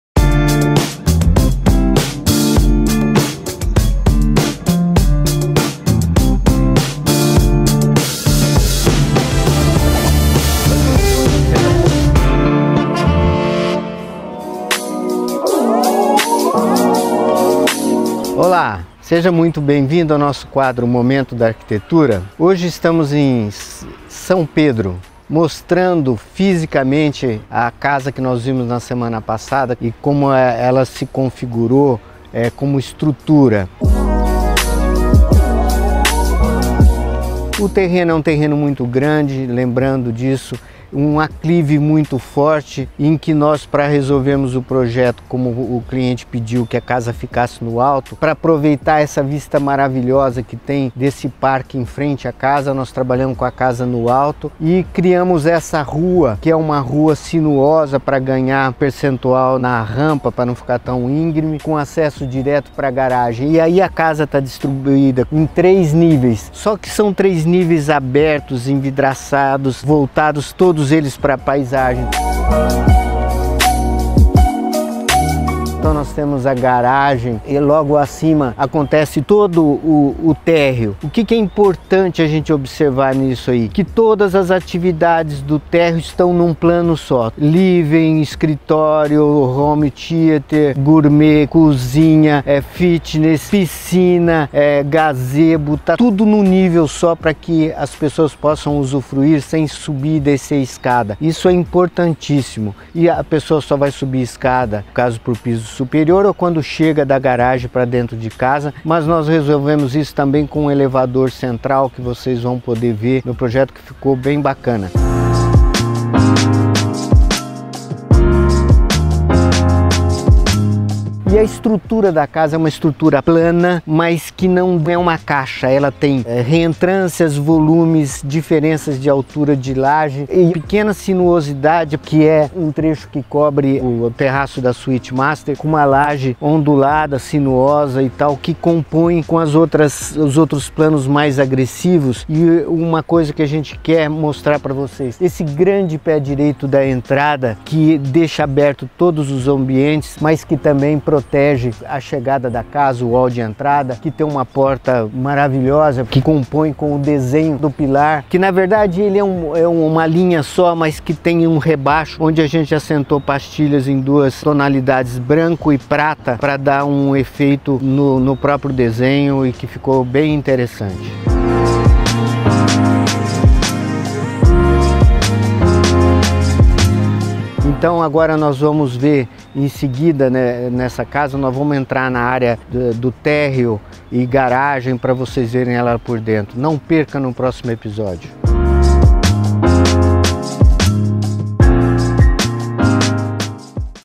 Momento da Arquitetura com Dreison Santini - Ep. 75 – Casa São Pedro – Arq. Externa
Casa em terreno de grande Aclive em São Pedro - SP - Arquitetura Externa